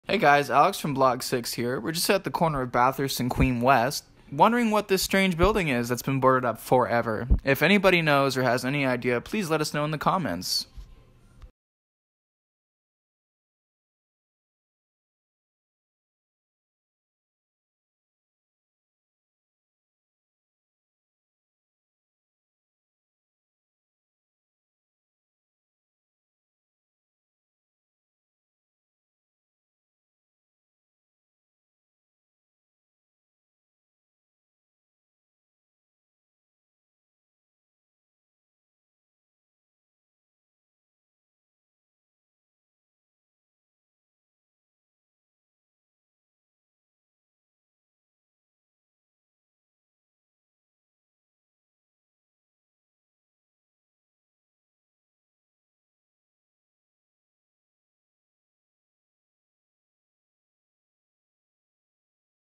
Explore Toronto: Galerie (Mystery Building!)
So this video is a bit different, we are asking YOU to tell us what the deal is with this building that has been boarded up since ... forever? Torontonians, drop us a comment to let us know!